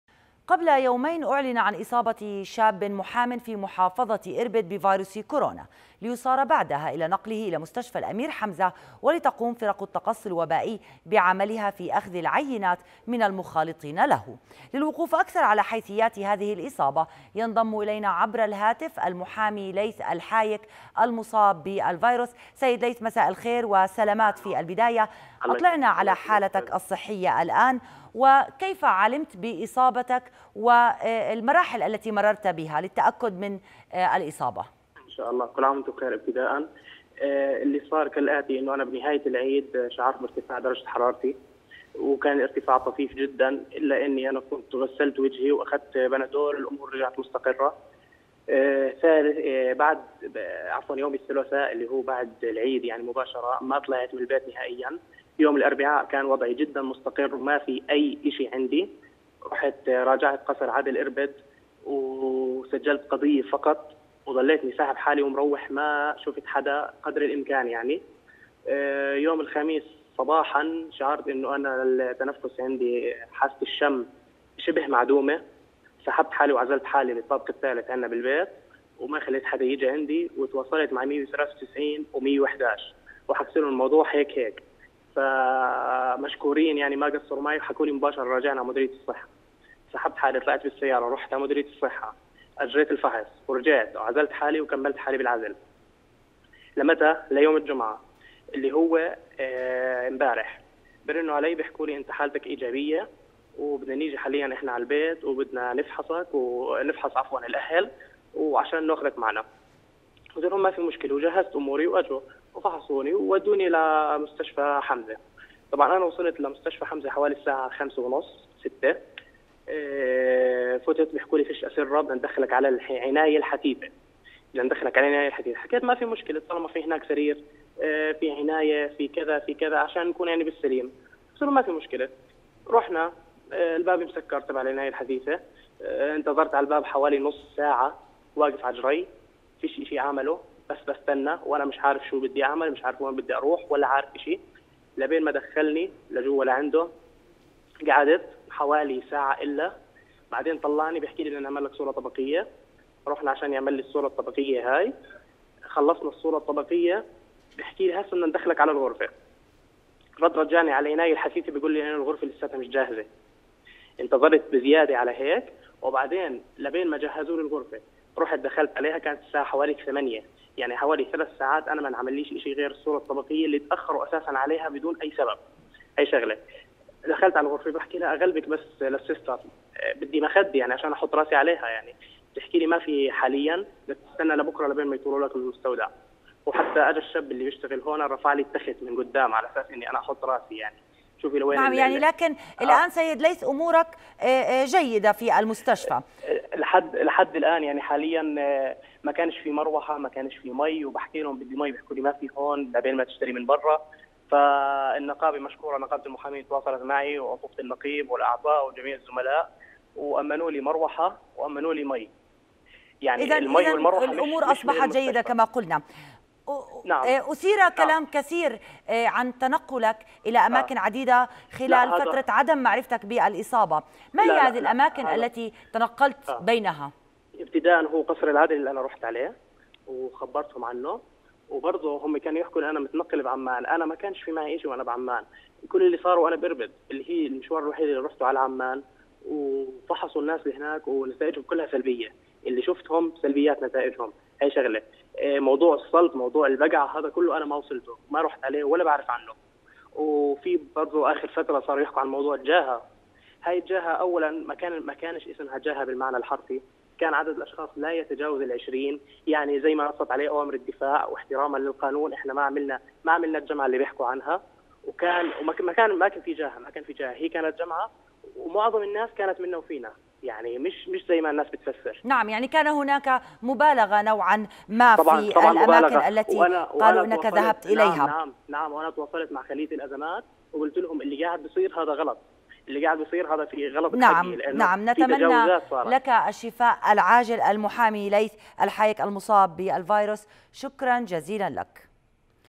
Irbid lawyer infected with COVID-19 speaks to Roya
The lawyer infected with coronavirus in Irbid has provided details about his initial symptoms and the decision to isolate himself.